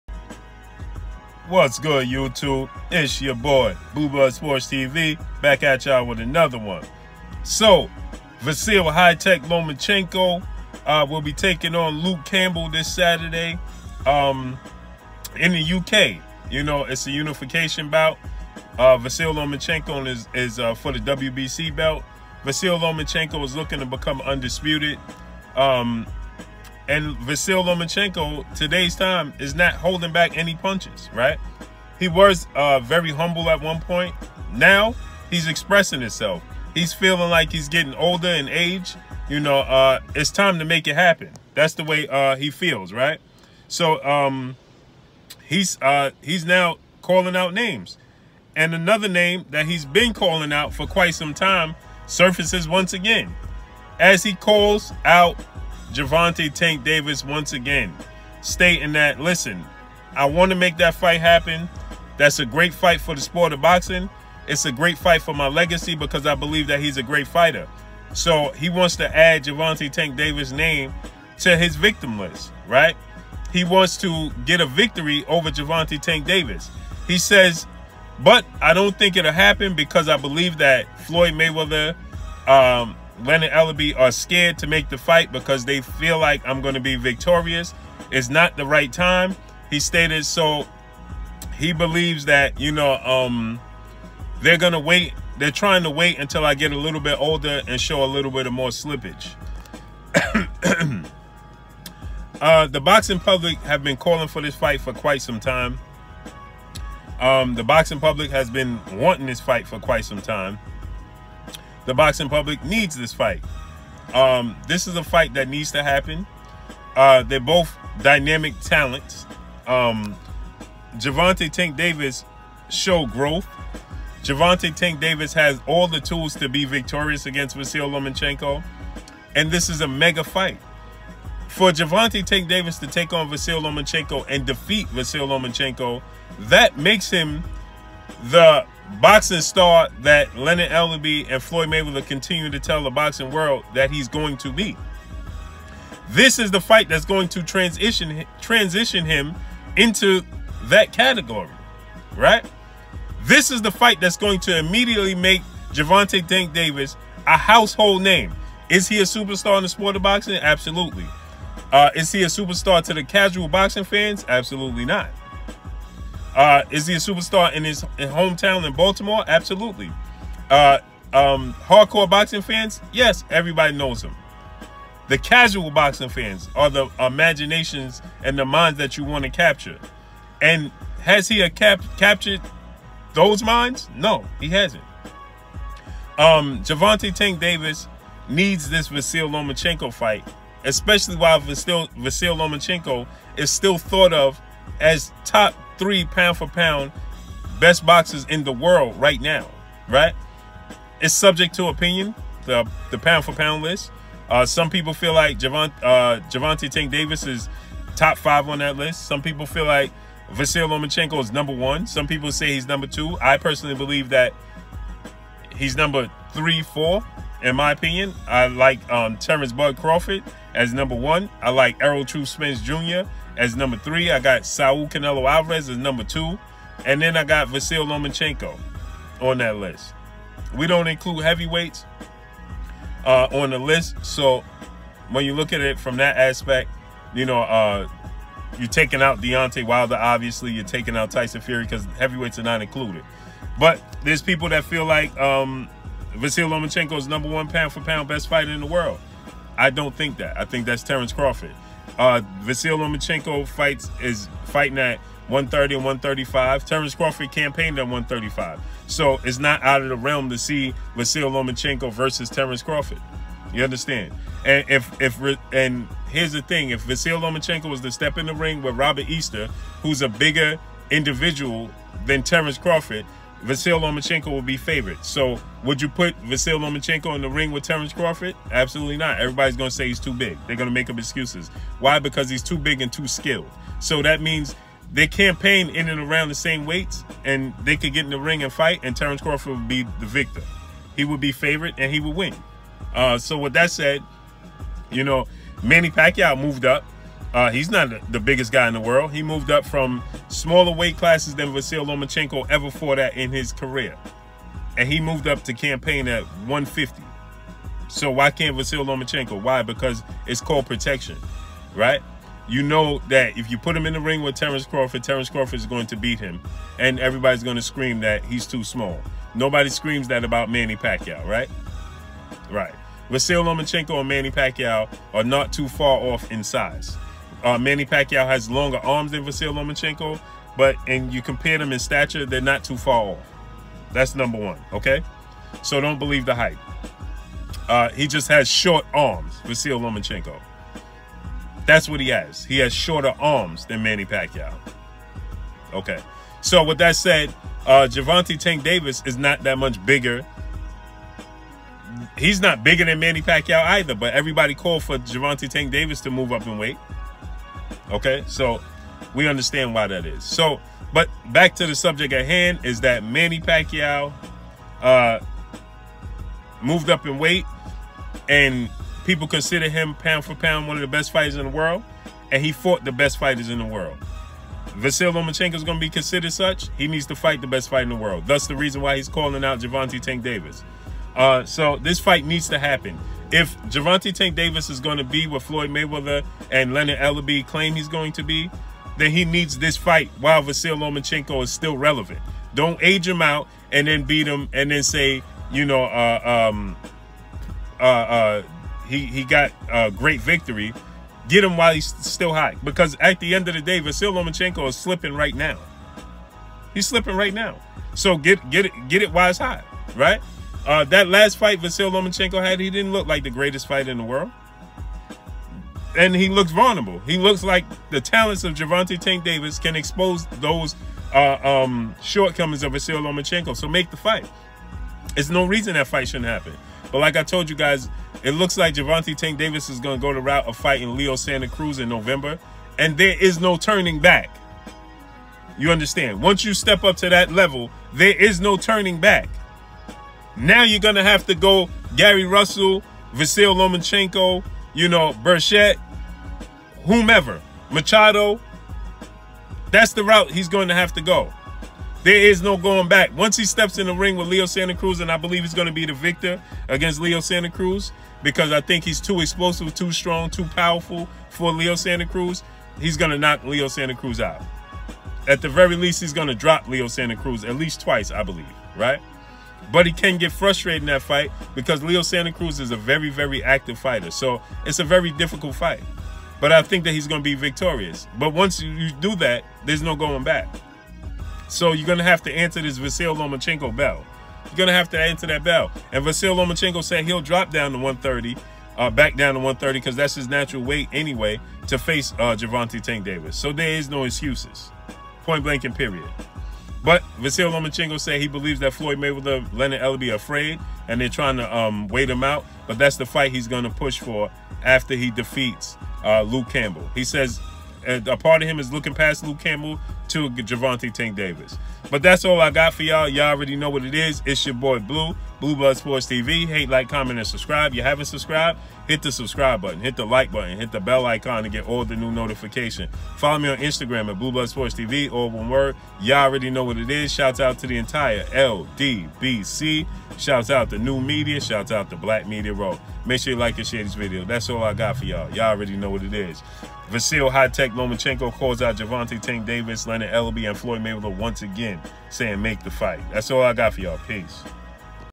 BREAKING NEWS: VASYL LOMACHENKO CALLS OUT FLOYD MAYWEATHER ! SCARED TO MAKE “TANK” DAVIS FIGHT !!
P4P BOXING SUPERSTAR THE MAN KNOWN AS “HI TECH” (VASYL LOMACHENKO) CALL’S OUT FLOYD MAYWEATHER AND HIS PROMOTIONAL COMPANY (MAYWEATHER PROMOTIONS) SAYING MAKE THE FIGHT BETWEEN MYSELF AND YOUR FIGHTER UNDEFEATED SUPERSTAR WBA LIGHTWEIGHT CHAMPION (GERVONTA “TANK” DAVIS) !! QUOTE: “ DON’T BE SCARED OR TRY TO AGE ME OUT” END QUOTE:

SOURCE: BLUE BLOOD SPORTS TV